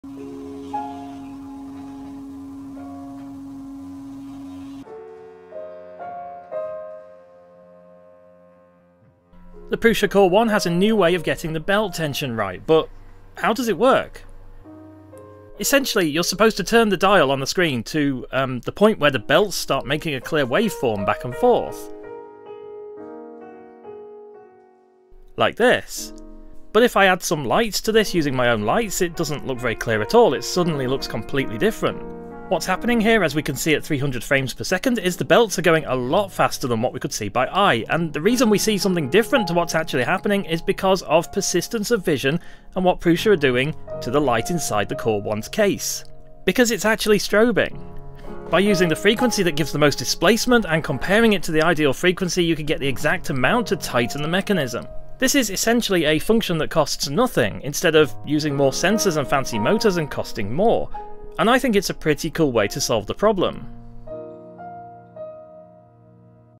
Prusa's new really cool belt tensioning function!
If you want to try this yourself you need to flash your firmware to the new 6.4 version or higher.